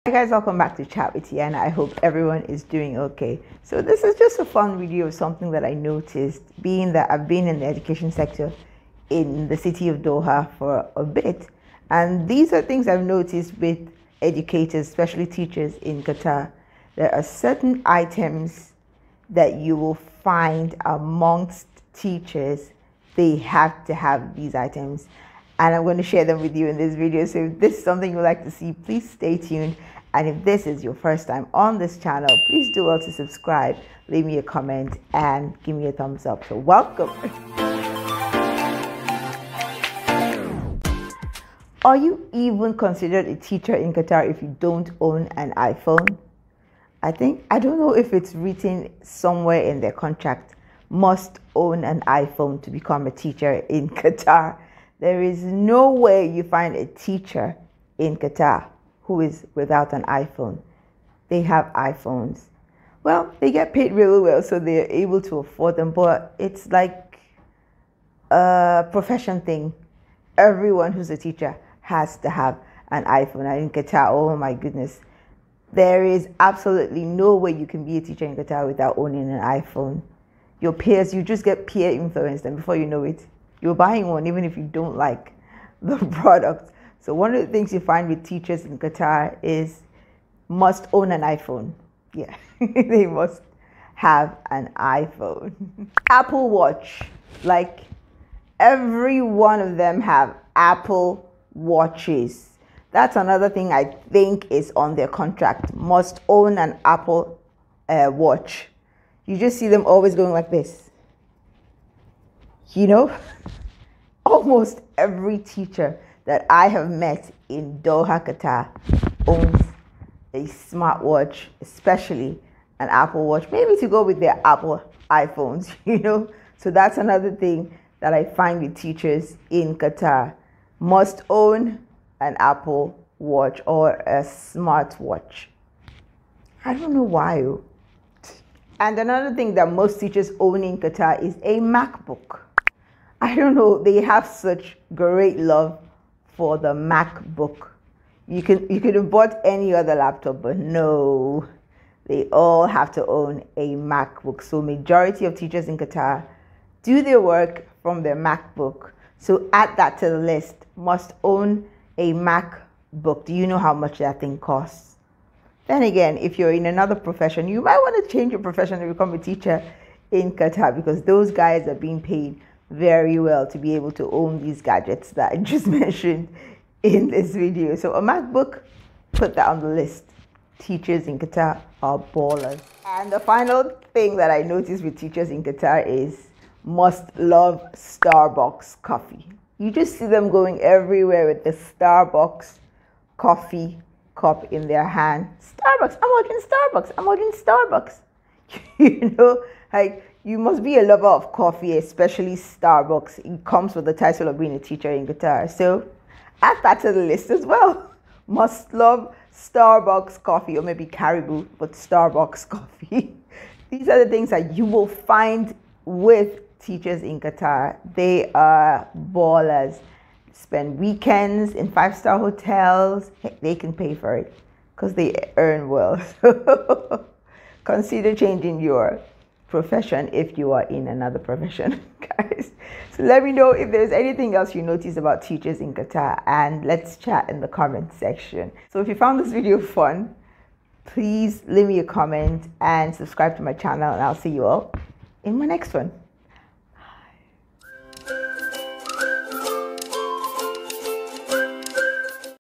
MUST HAVE ITEMS FOR TEACHERS IN QATAR
Find out the luxury items that Teachers in Qatar splurge on.